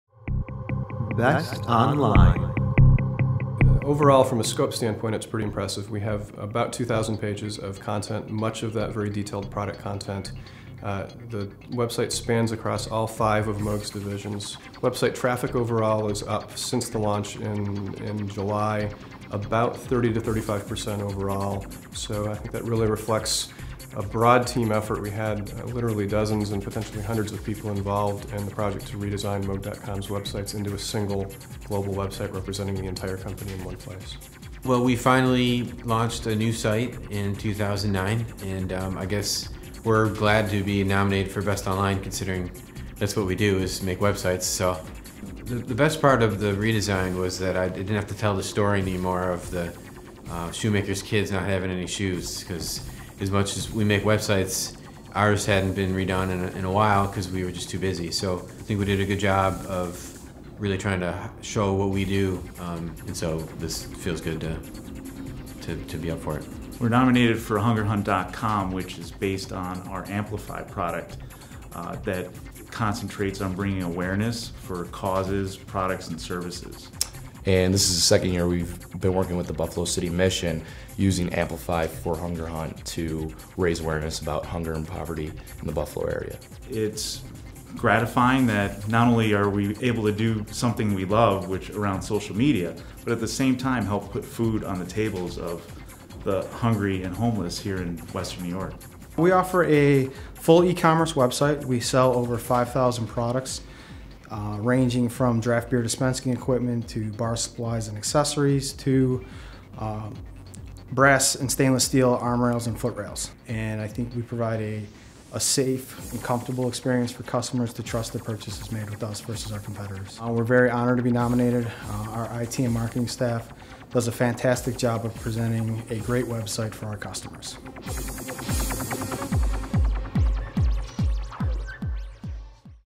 Best Online
Best Online is an award for local companies that have launched a new website in the last year, and their web presence resulted in increased business & visibility for both your company and your customers company.